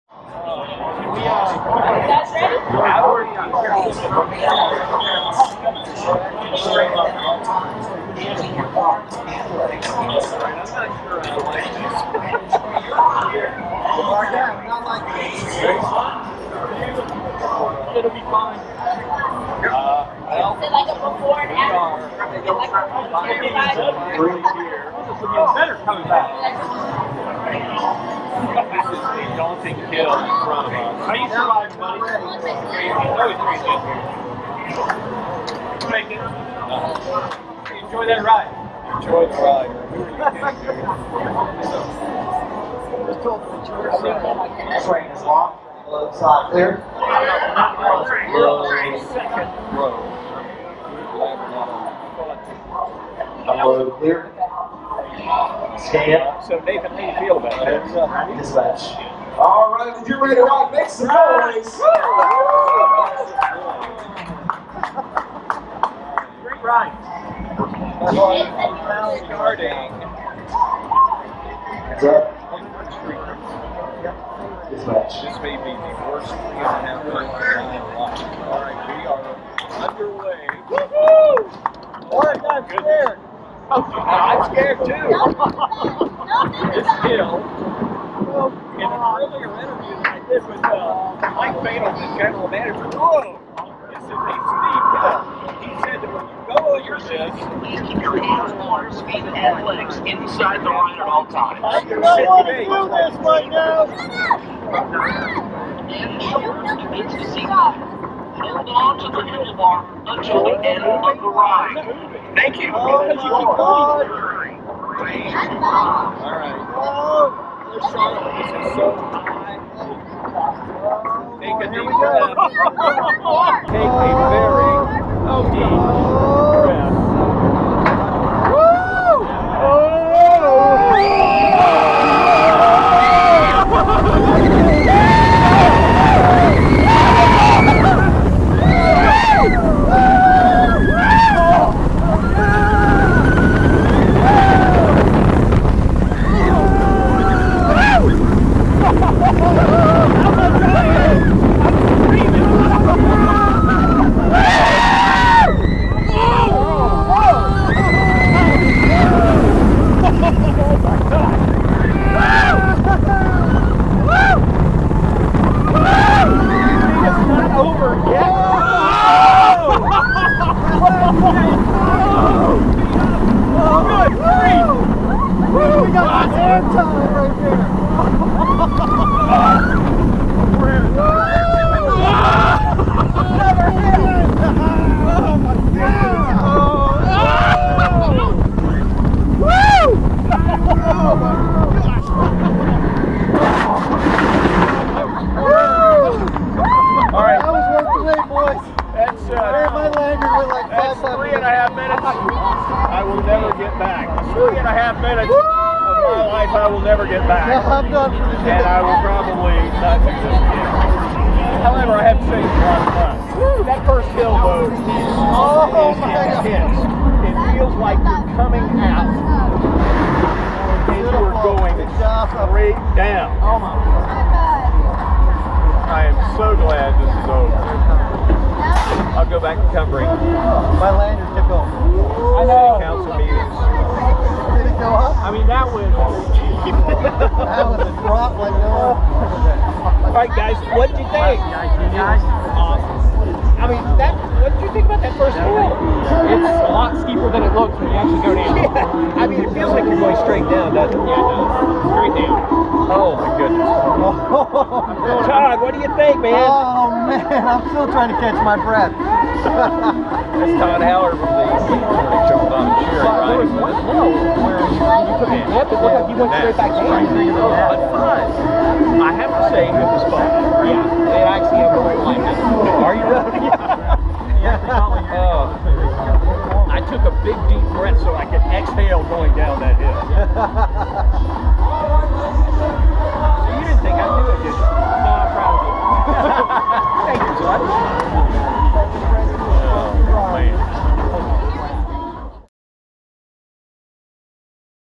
Super Todd On The New Fury 325
107.9 The Link's Super Todd rides Carowinds' new Fury 325!